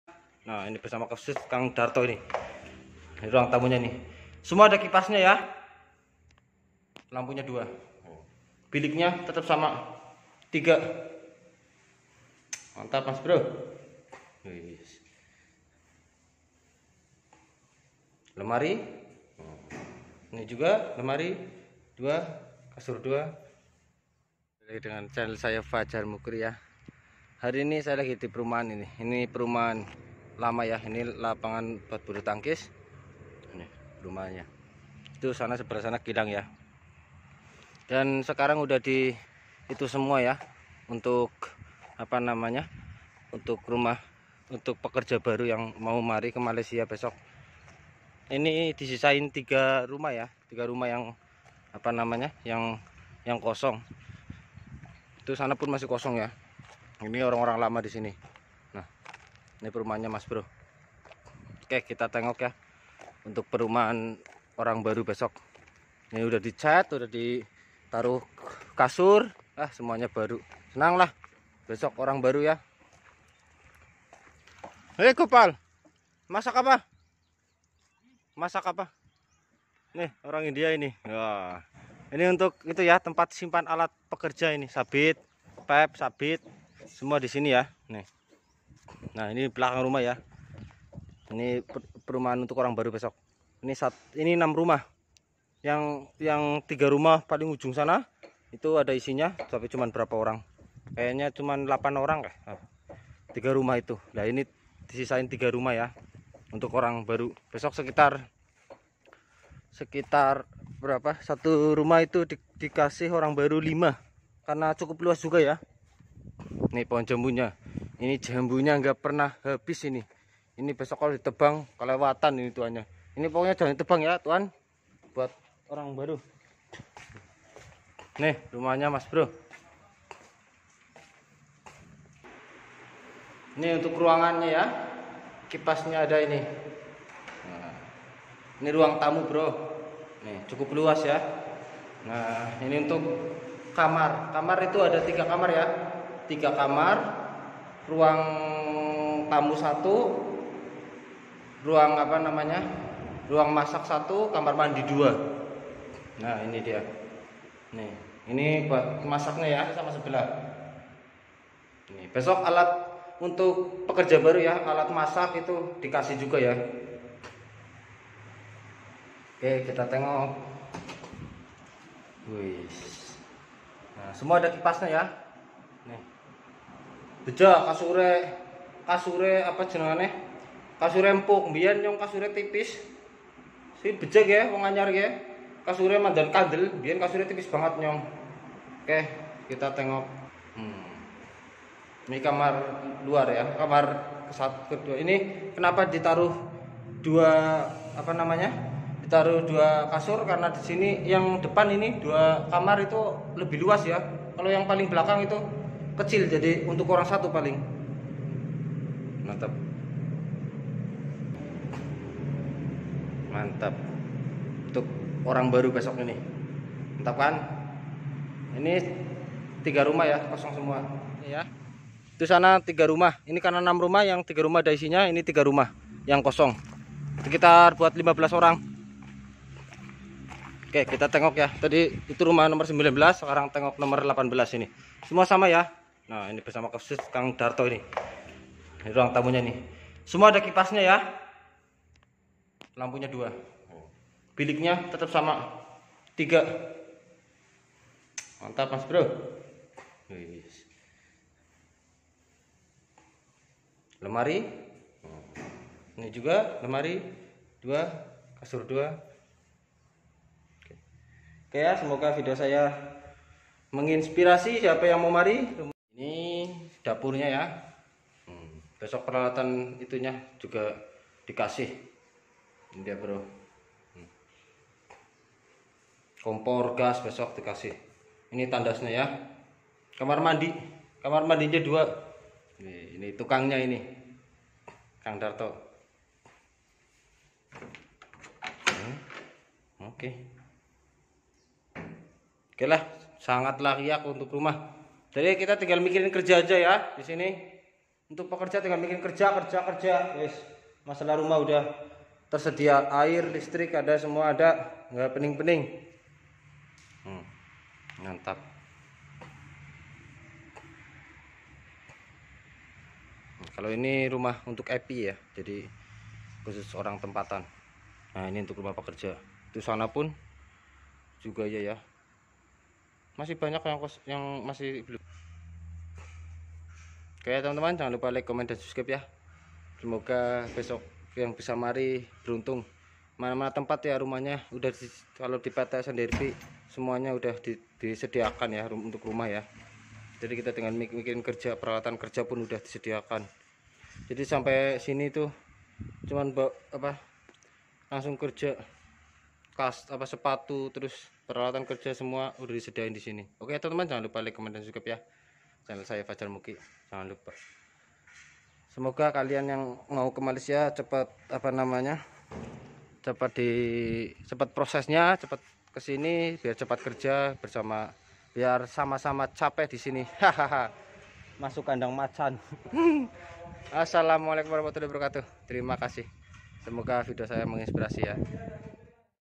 RUMAH UNTUK PEKERJA BARU || TAPI ORANG BARUNYA BELUM DATANG HHH
rumah untuk pekerja baru di perkebunan sawit dimalaysia rumahnya cukup layak yah
tinggl nunggu orang barunya